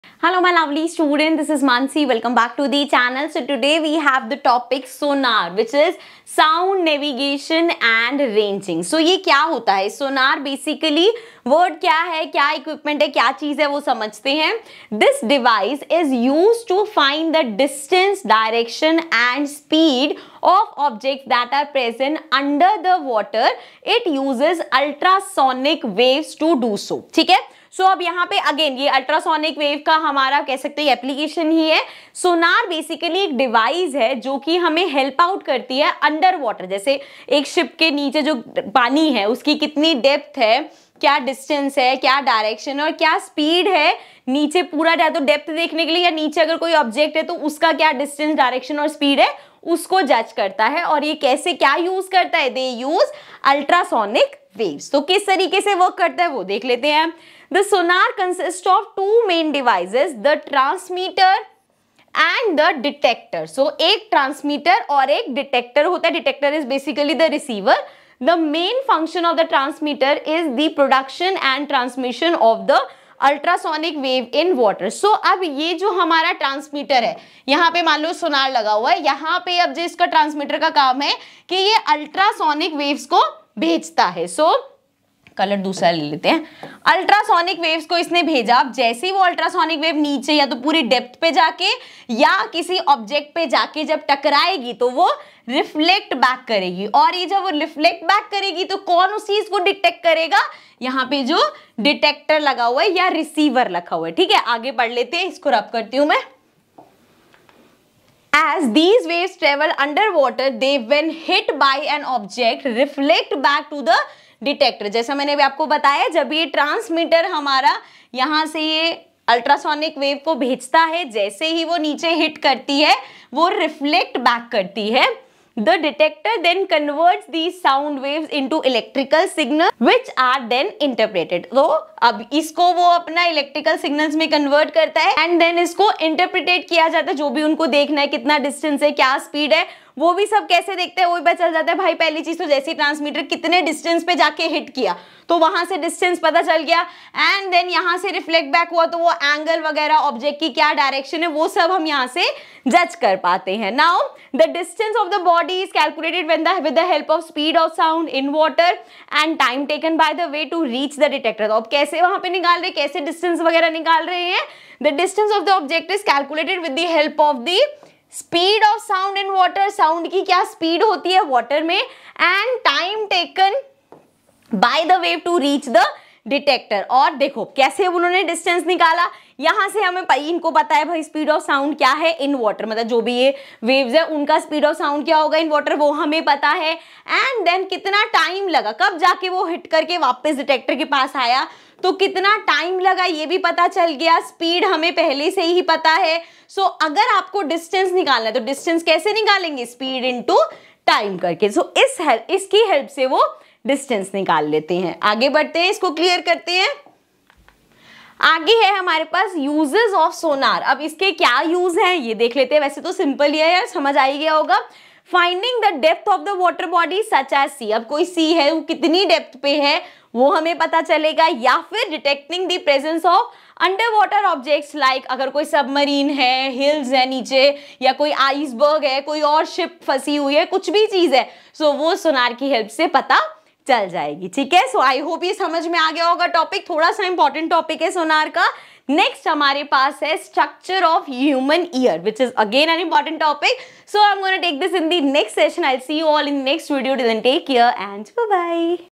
SONAR | Sound Navigation and Ranging | Chapter 12 | Class 9 Science
In this session we will be continuing with the chapter "Sound".
We will cover SONAR of chapter 12.